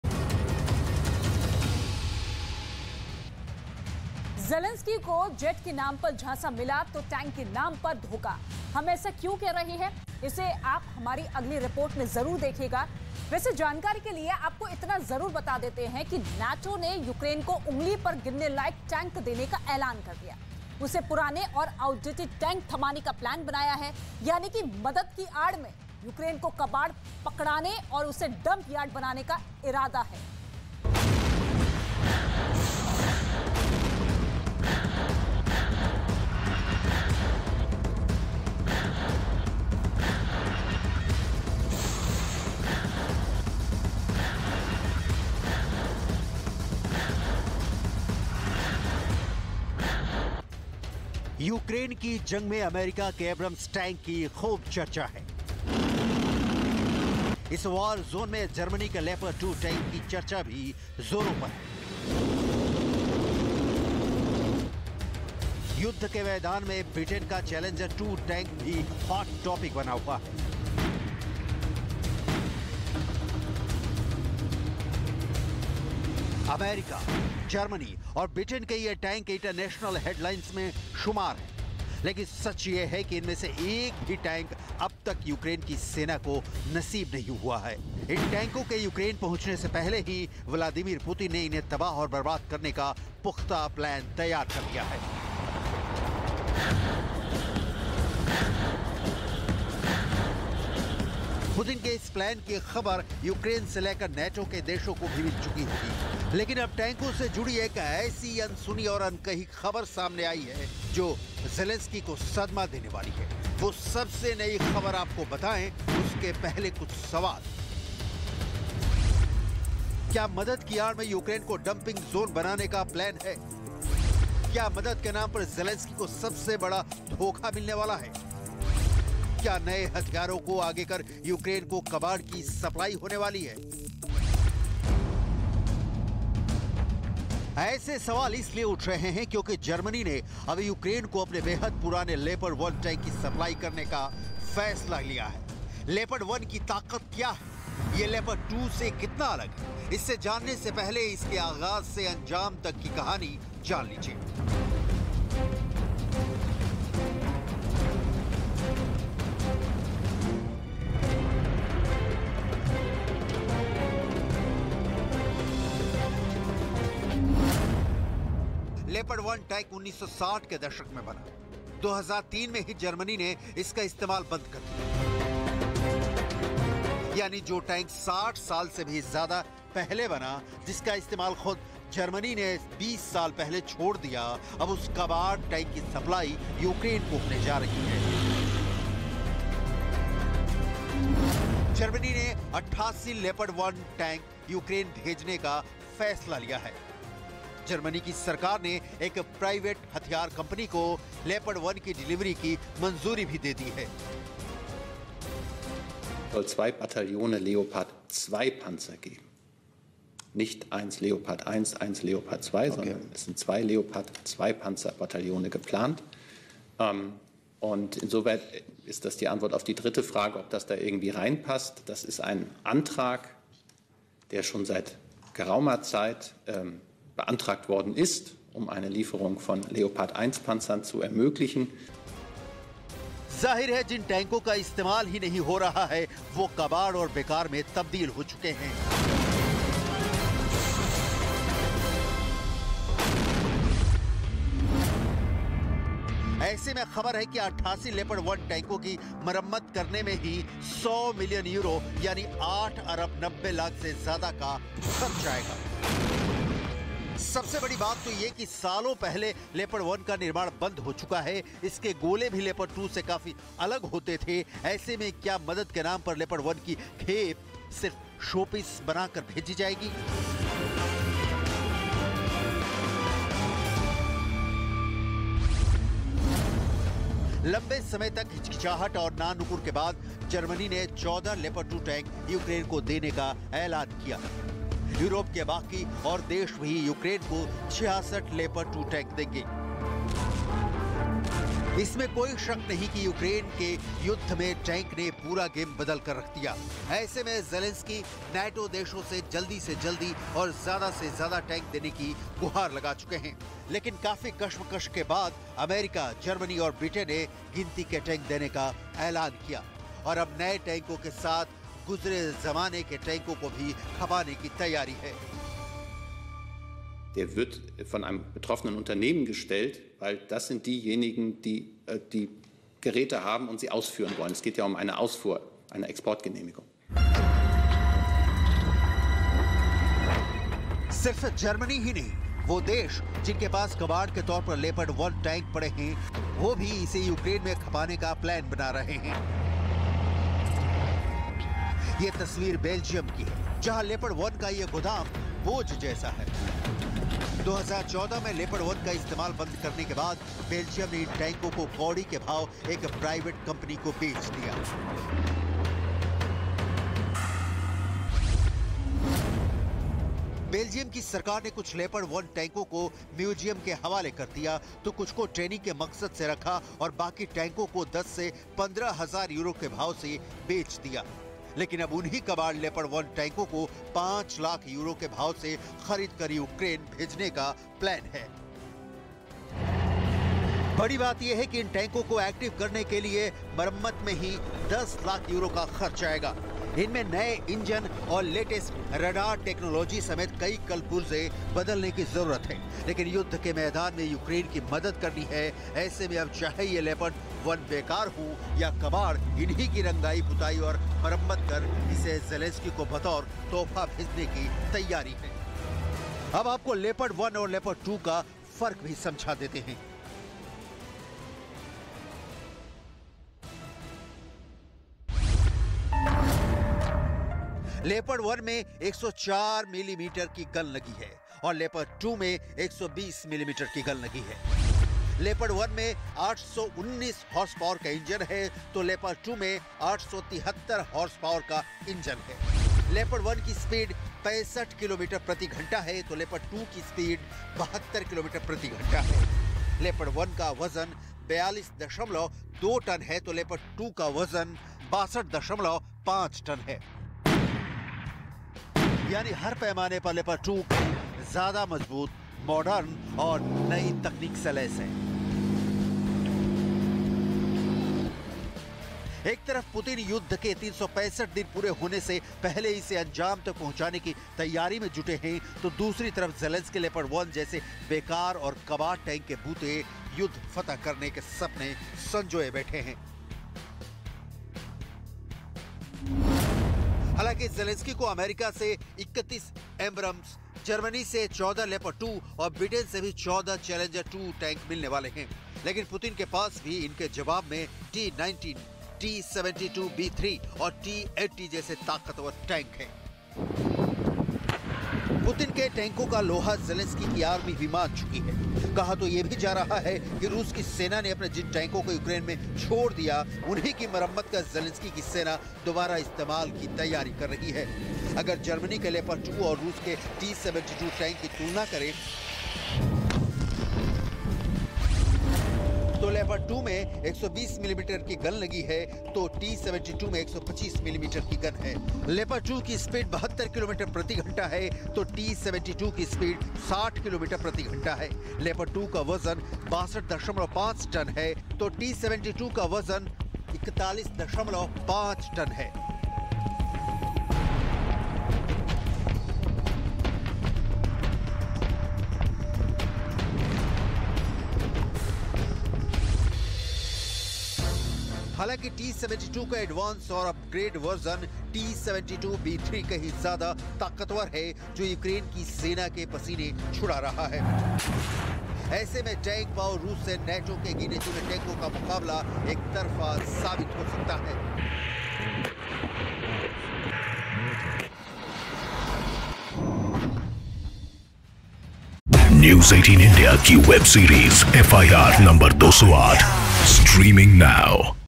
यूक्रेन बन गया 'डंपिंग यार्ड', युद्ध में Zelenskyy से 'धोखा' हुआ | Ukraine Russia War | Hindi News
ज़ेलेंस्की को जेट के नाम पर झांसा मिला तो टैंक के नाम पर धोखा..हम ऐसा क्यों कह रहे हैं? इसे आप हमारी अगली रिपोर्ट देख कर पूरी तरह समझ जाएंगे..वैसे जानकारी के लिए आपको इतना बता देते हैं कि NATO ने यूक्रेन को उंगली पर गिनने लायक टैंक देने का ऐलान कर..उसे पुराने और आउटडेटेड टैंक थमाने का प्लैन बनाया है..यानी मदद की आड़ में यूक्रेन को कबाड़ पकड़ाने और उसे डंप यार्ड बनाने का इरादा है..